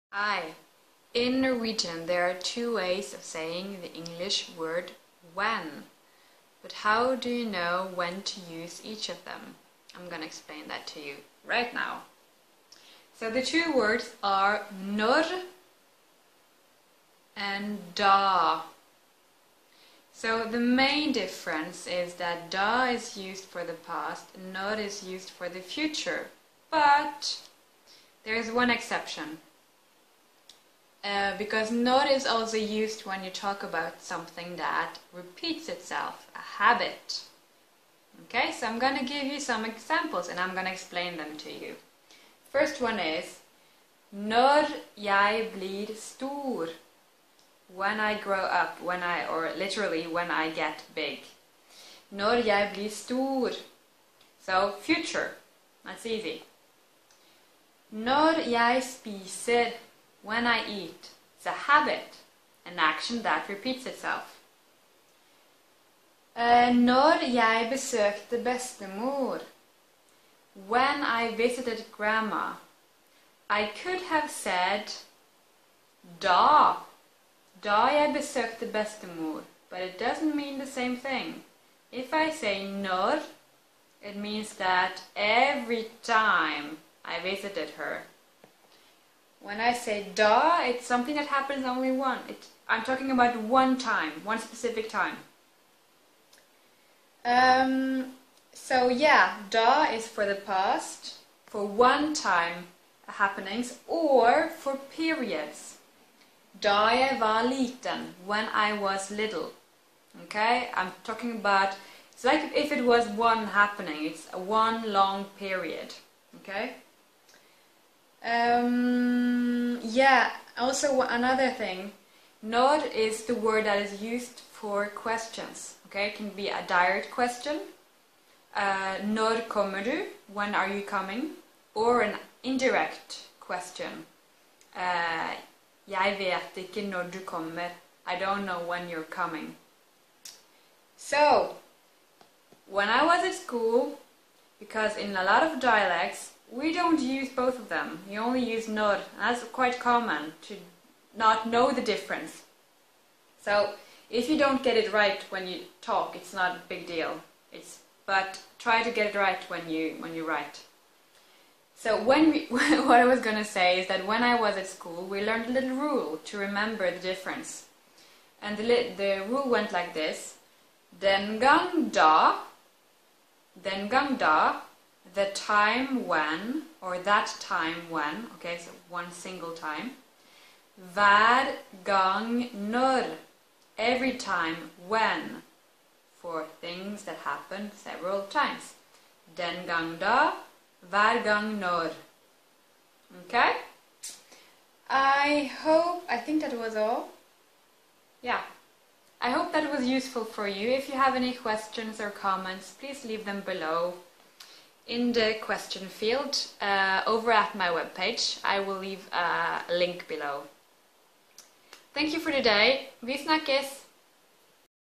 "Da" and "når" in Norwegian
The two Norwegian words "da" and "når" can both be translated into "when", so how do you know when to use each of them?

Vi snakkes!